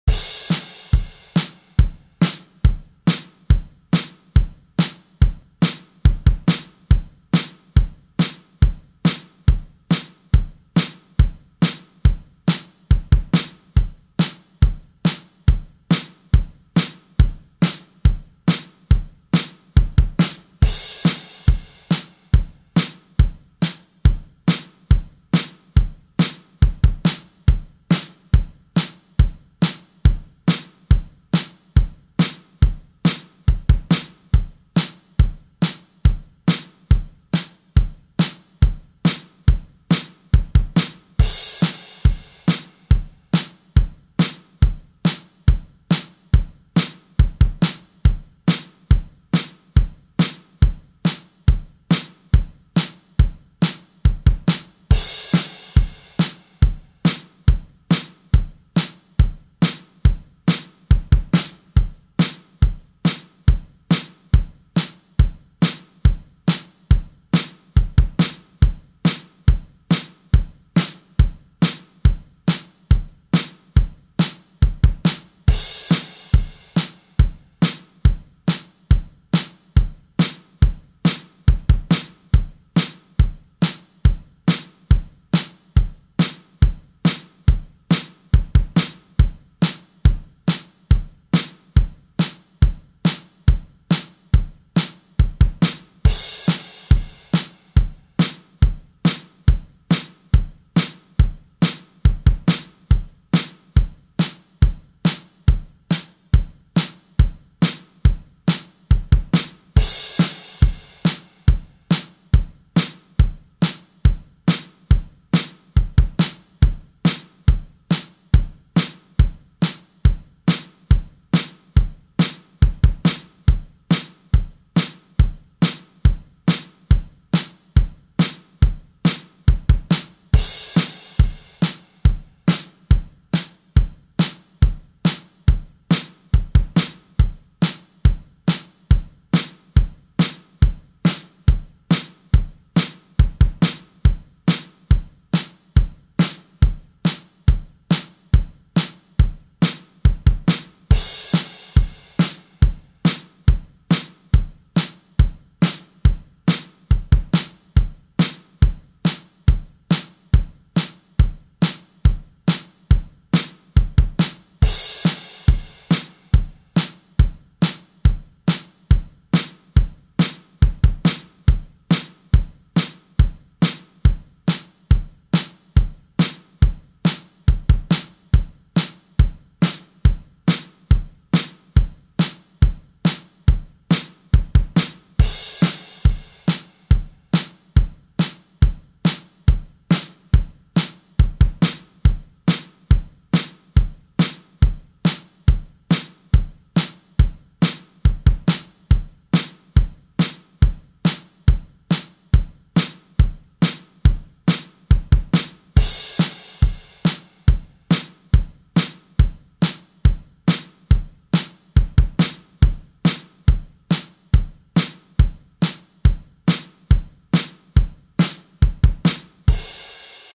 140 BPM BASIC 4:4 ROCK BEAT
128 Measures of Basic Rock Drums for your to practice or Jam to!